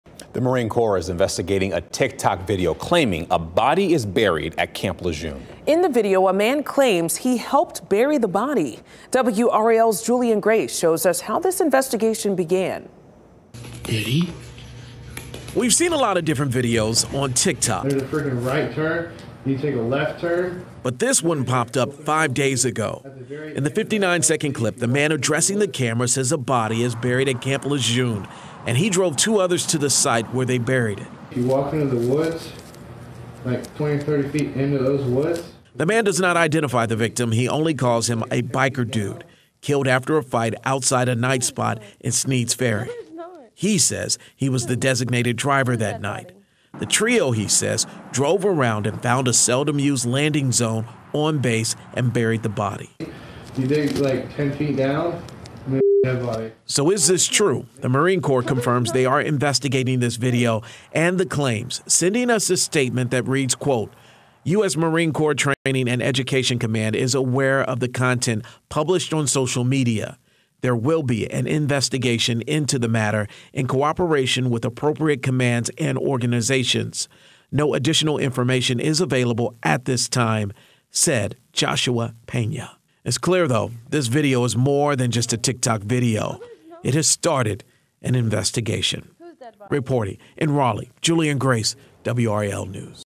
Marines, NCIS investigating vidoe saying body was buried at Camp Lejeune
A TikTok video claims a marine helped bury a body somewhere on the base, prompting an investigation by the Marines and NCIS.